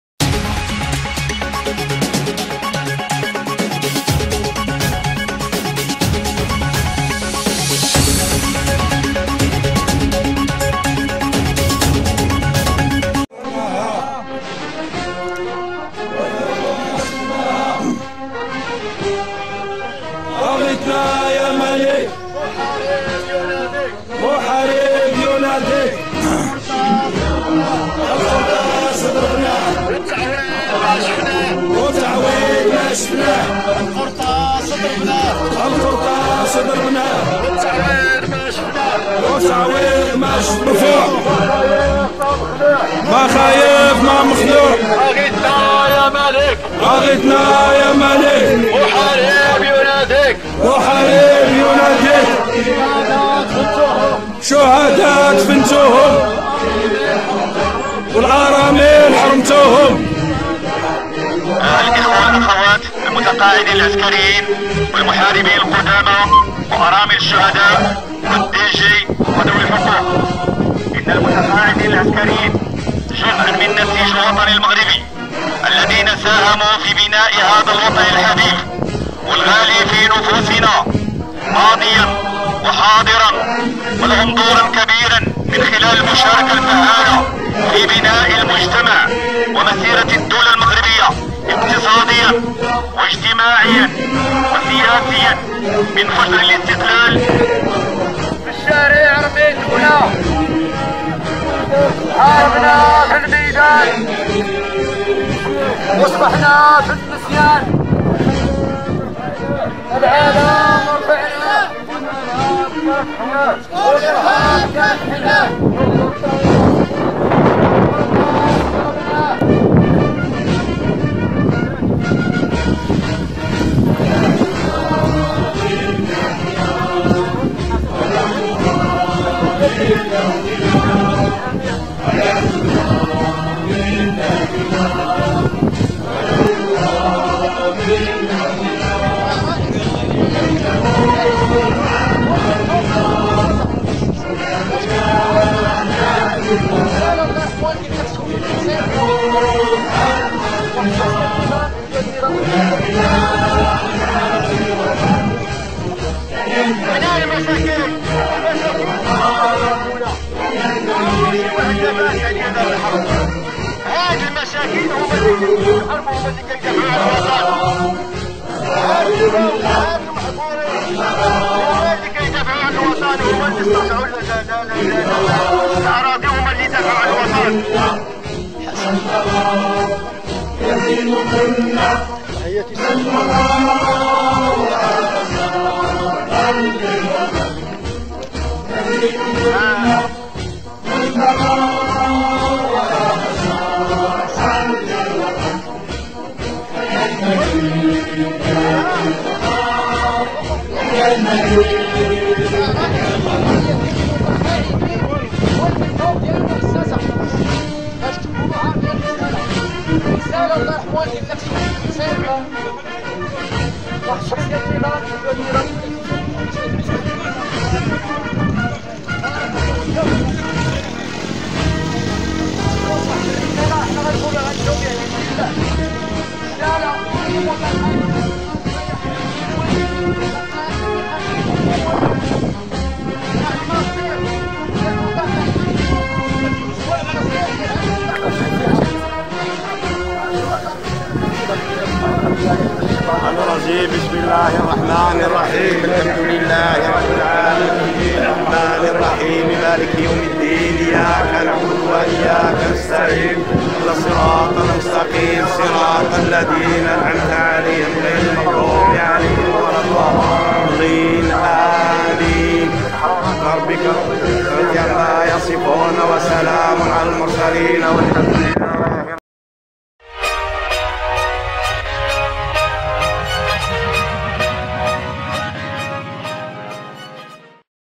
قدماء المحاربين والمتقاعدين وارامل الشهداء يناشدون ....
قدماء المحاربين والمتقاعدين وارامل الشهداء يناشدون الملك بشعارهم ~ اغثنا يا ملك~ في مسيرتهم السلمية...شعارهم الله---الوطن---الملك.